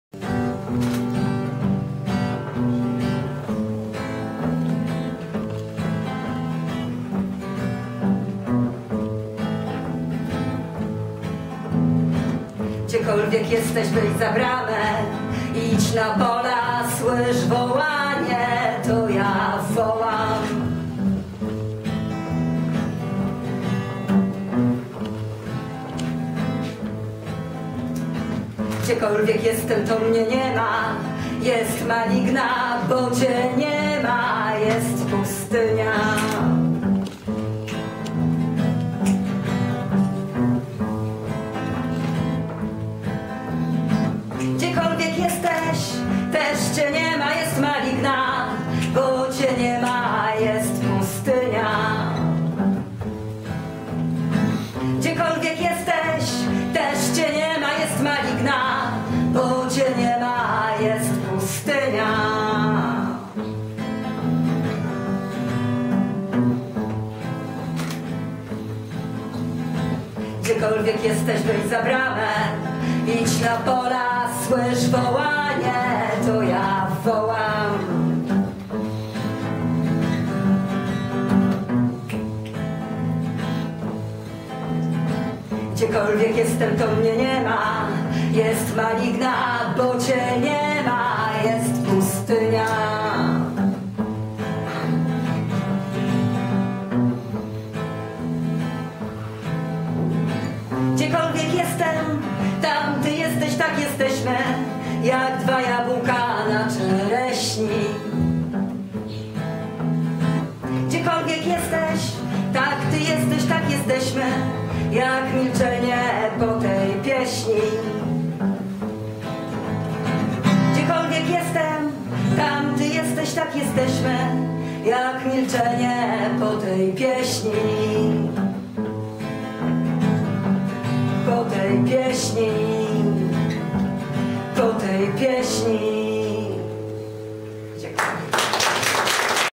sted w 75 urodziny w rumskim domu kultury
sted w 75 urodziny w rumskim domu kultury w wykonaniu izy i janusza